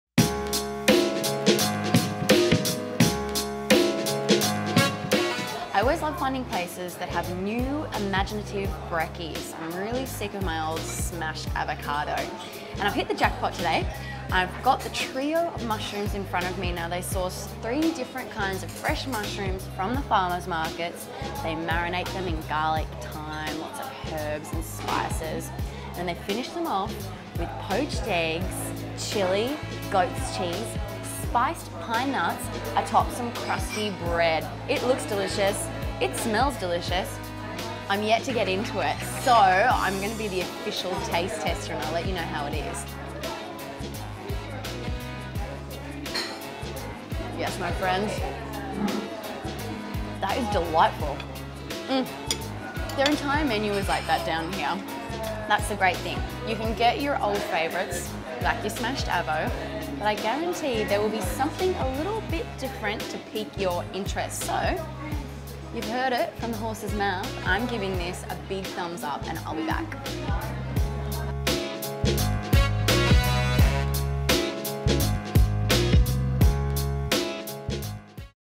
Miss Marmalade a Coffee Shop in Melbourne serving Coffee and Salad
Experience great tasting coffee, superfood salad, awesome atmosphere and services here in Miss Marmalade.

Location: 126 Union Street Brunswick VIC 3056